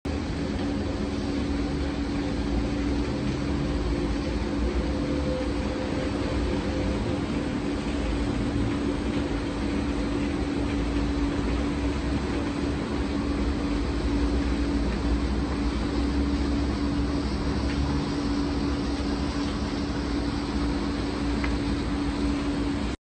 new westminster pier fire. Terrible
Video of the Pier fire going on in New Westminster BC right now 👎